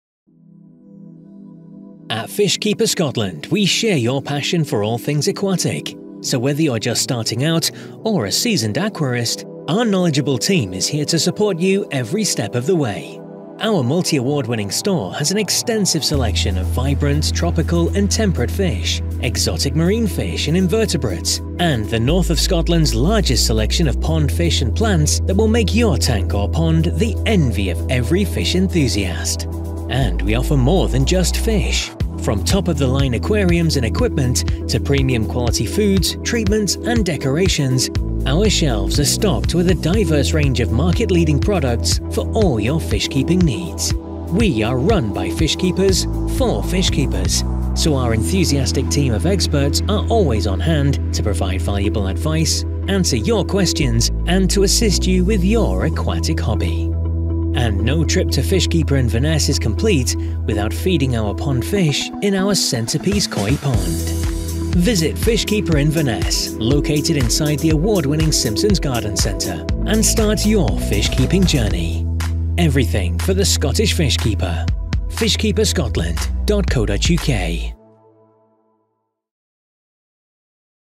Fishkeeper Inverness
Located within the garden centre is Fishkeeper Inverness, with a wealth of knowledge and products for the fish keeper in the Highlands. Whether you're just starting out on your fish keeping journey or a complete pro, the team have everything you need for setting up or maintaining your cold and warm water tanks. Visit the concession today, which is open the same hours as the garden centre.